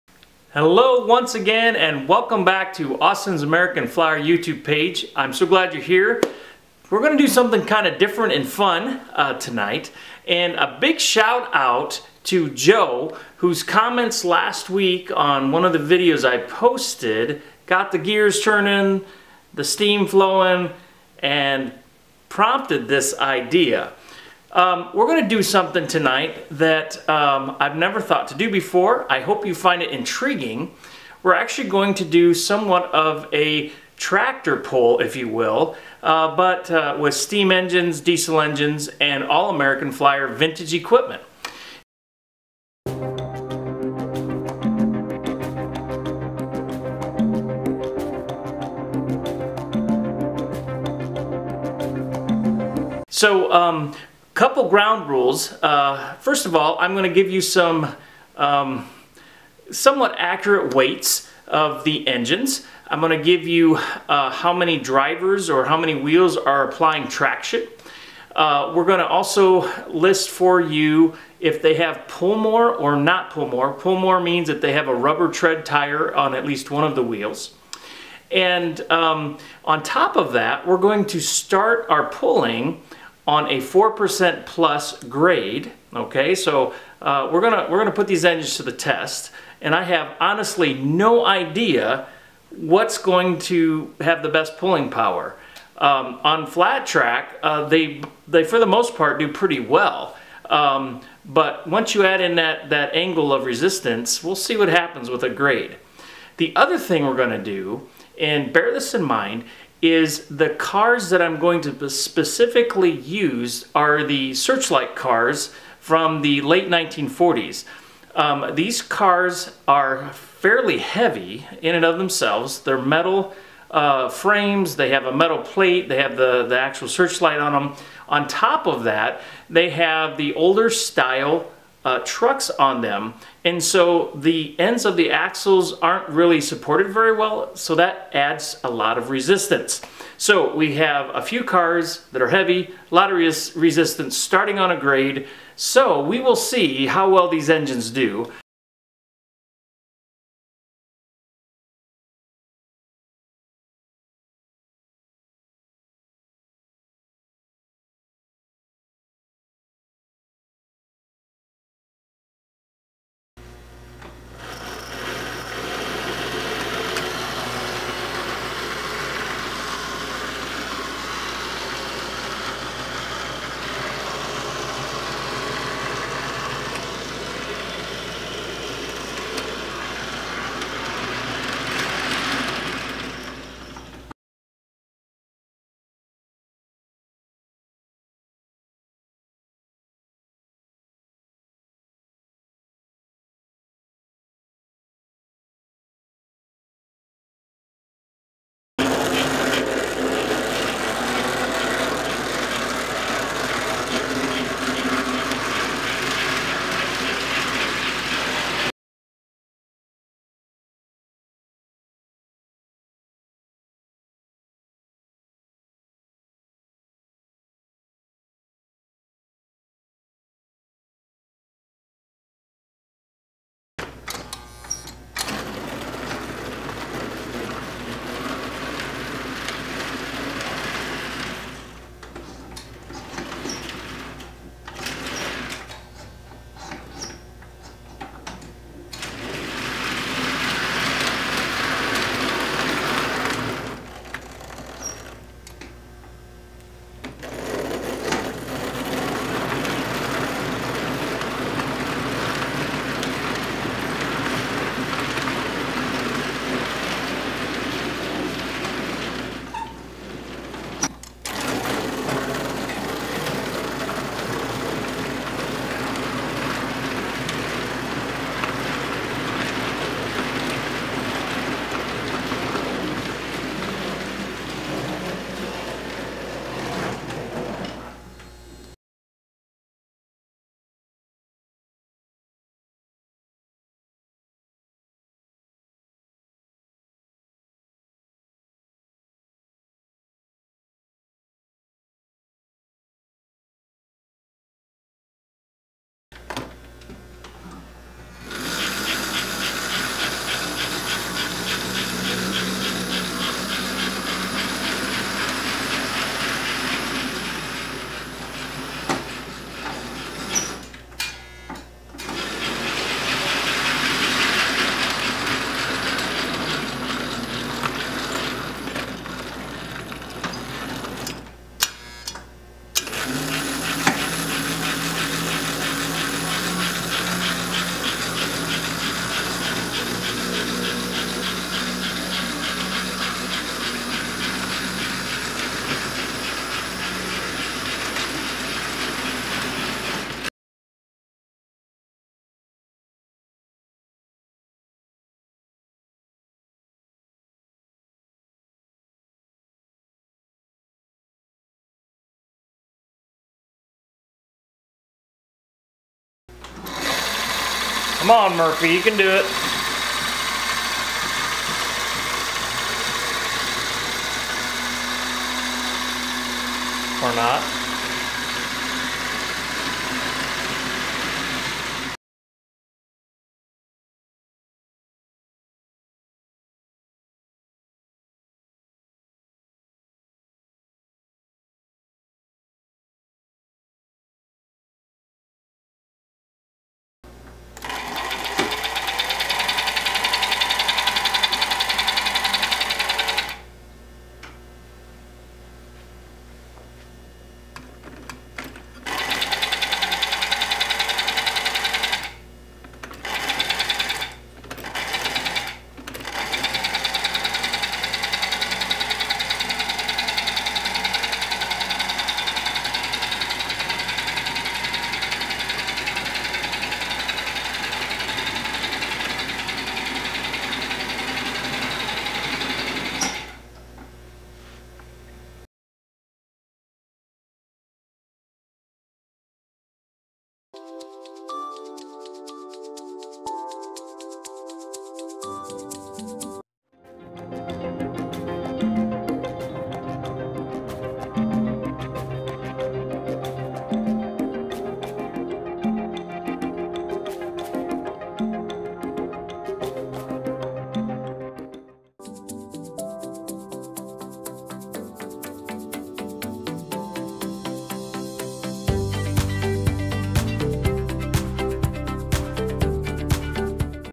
'I think I can, I think I can!' Part I: How well do various vintage American Flyer engines pull?
Testing the tractive effort or pulling power of various vintage American Flyer engines.

More Flyer fun on instagram!  Find me @  Americanflyertrainfun